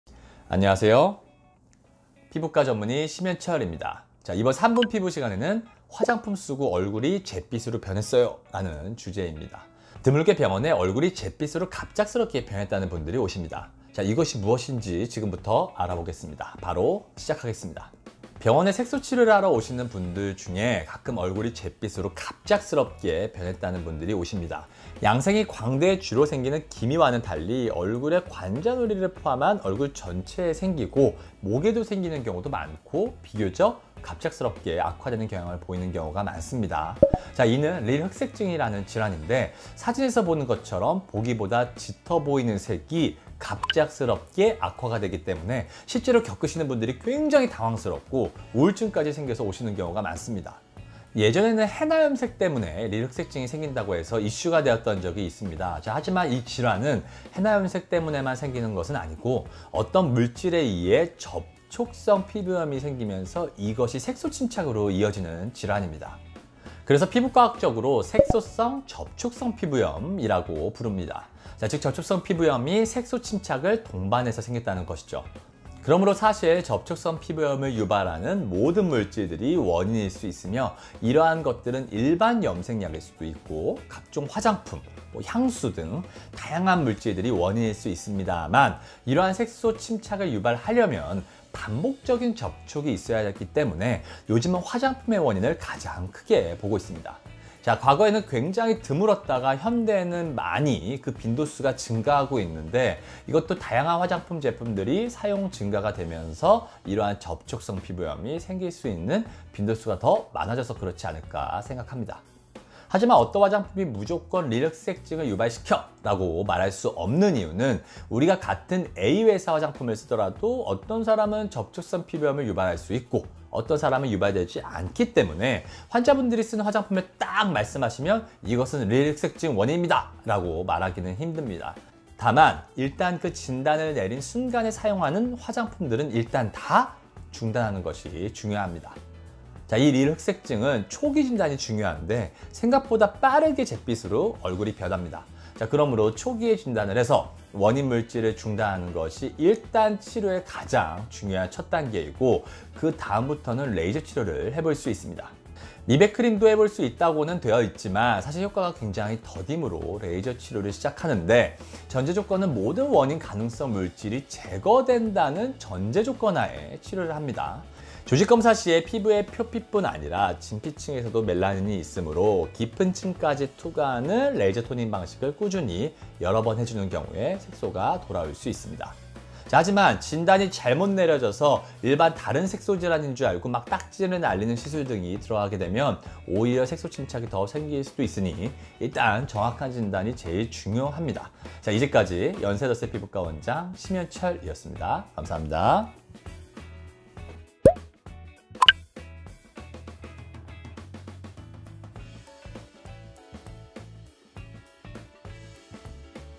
3분 피부꿀팁 화장품 잘못써서 얼굴 잿빛된 사연 피부 하얘지는법 [피부과전문의 피부심]
이번 3분 피부관리 꿀팁에서는 화장품을 잘못써서 얼굴이 잿빛으로 변한 경우에 대해 알아보겠습니다. 병원에 오시는 분 중에는 얼굴이 잿빛으로 갑작스럽게 변했다는 분들이 의외로 많습니다. 이렇게 피부가 뒤집어졌을 때 얼굴 피부 하얘지는 법이 필요하겠죠? 이것이 무엇인지 지금부터 알아보겠습니다.

*영상순서
▶릴흑색증이란?